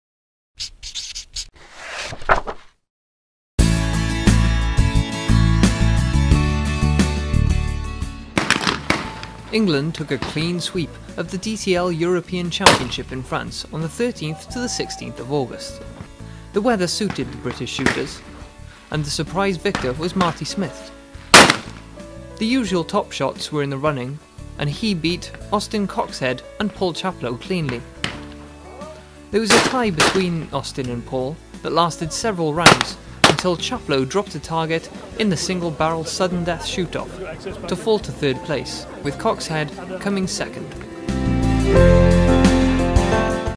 Team England dominate the DTL European Championship in France
Watch coverage of the DTL European Championship that took place on 15-16 August at Dubourdieu Castle Shooting Ground in France, where England’s top shots took a clean sweep of the podium places.

For a full report read the September edition of Clay Shooting magazine.